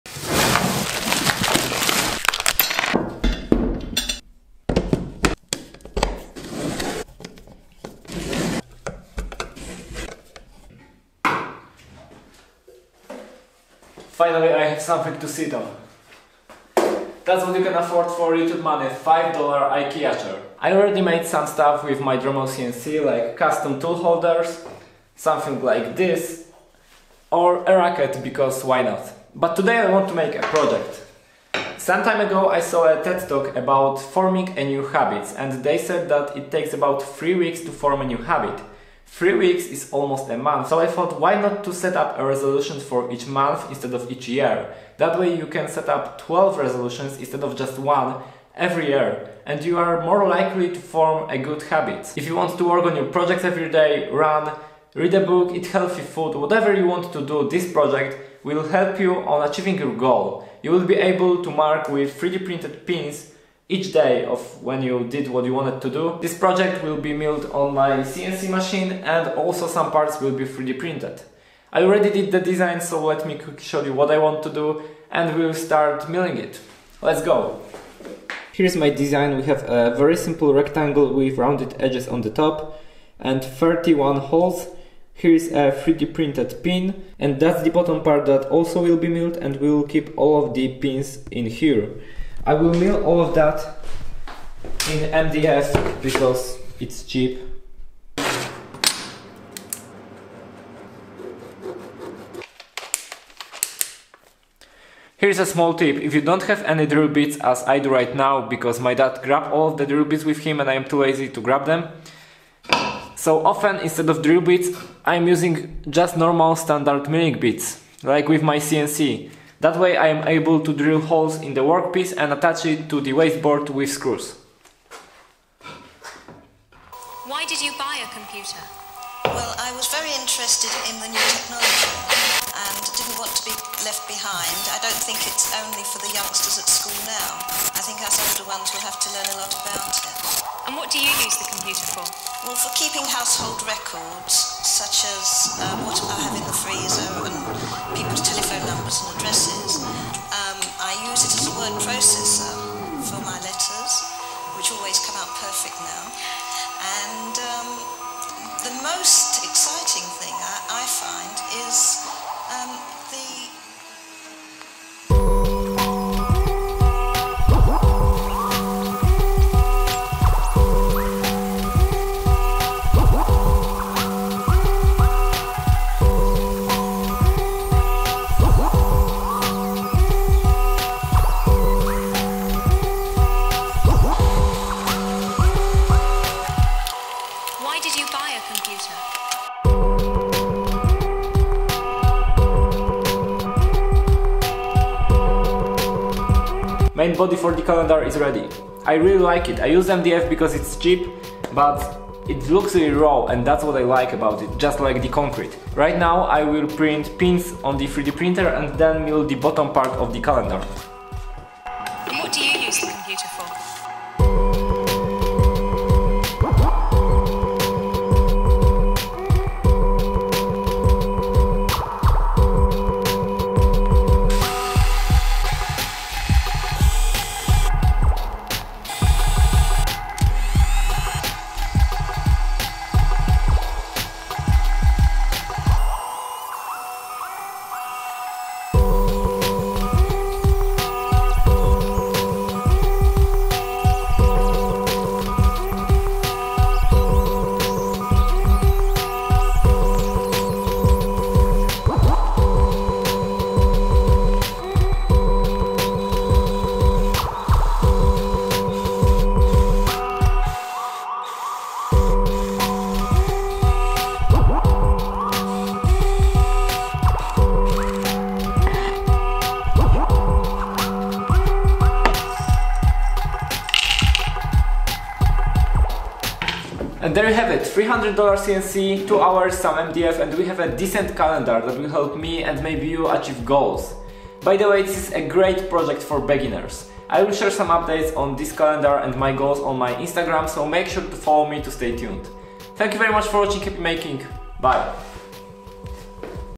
Goal Calendar - DIY Dremel CNC project
Finally, I made a complete project using DIY Dremel CNC! It's a really simple calendar that will help you to achieve your goals and also let you learn some CNC machining. To keep it cheap I used MDF, but it turned out that raw look of MDF really looks nice! I also 3D printed those green pins but you can really make them out of anything, you can even use M8 screws for that :)